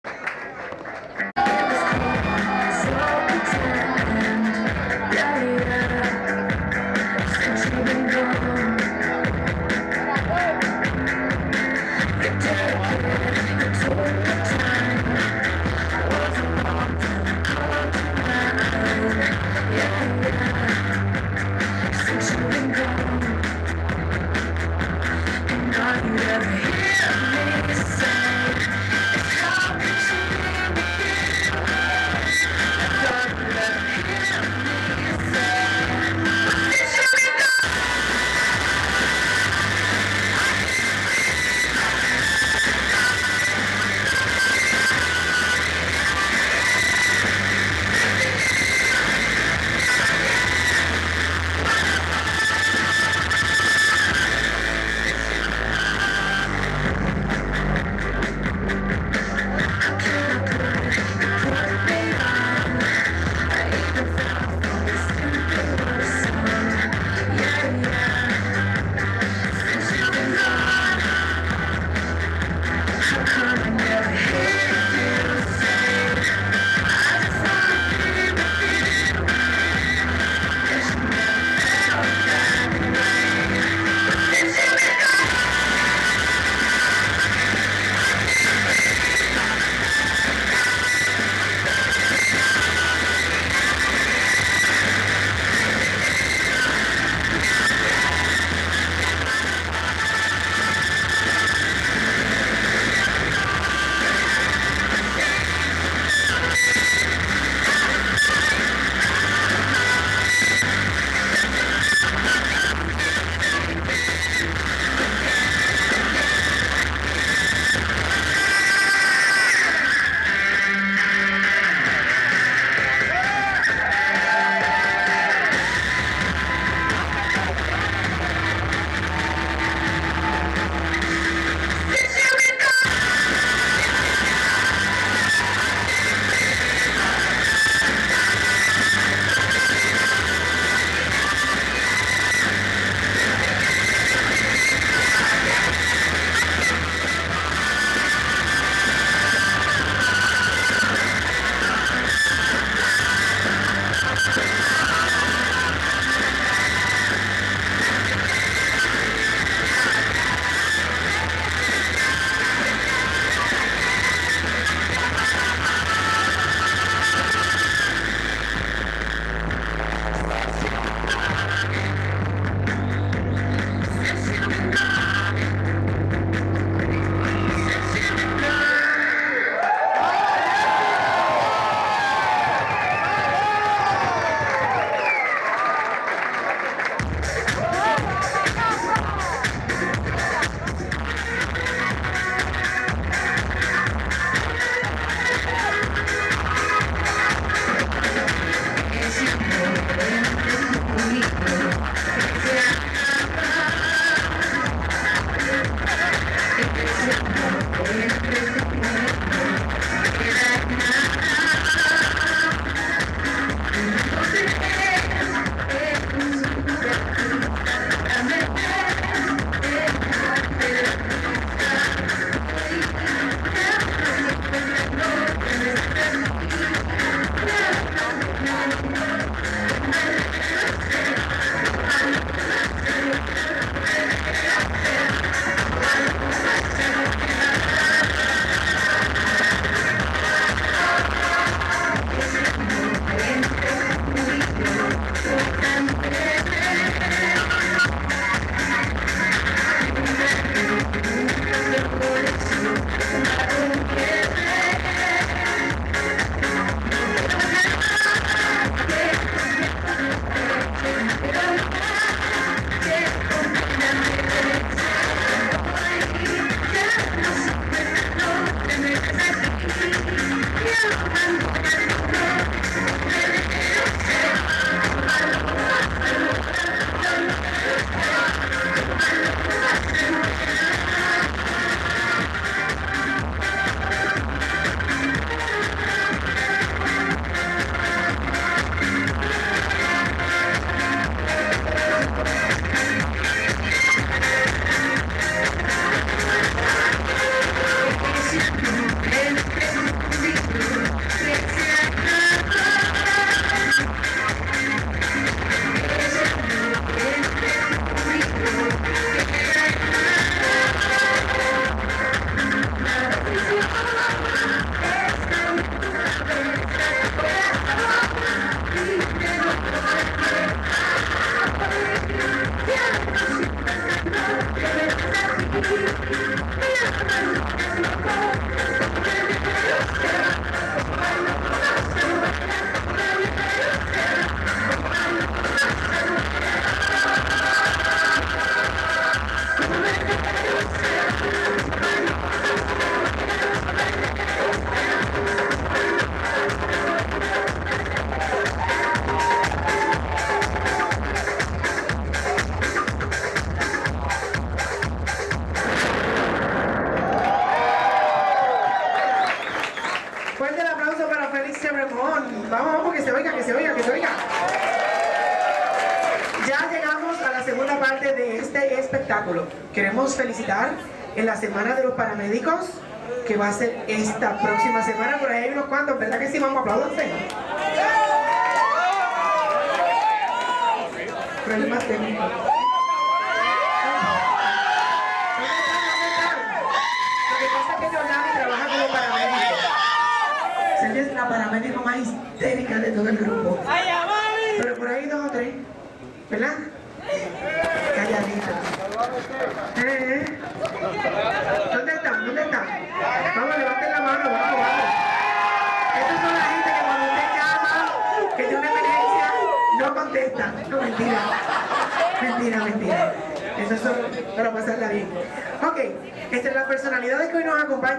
(((JESSICA WILD & FELIX CHEVREMONT)))..."NUESTRA BELLEZA LATINA PLUS"
LA DIVA,JESSICA WILD Y EL CAMALEON,FELIX CHEVREMONT  EN LA CORONACION DE JULIANYS COMO NUESTRA BELLEZA LATINA GAY PLUS.

JESSICA WILD & FELIX CHEVREMONT ON JULIANYS LEBLANCHE'S CROWNING NIGHT AS "LATIN BEAUTY GAY PLUS"...AT KRASH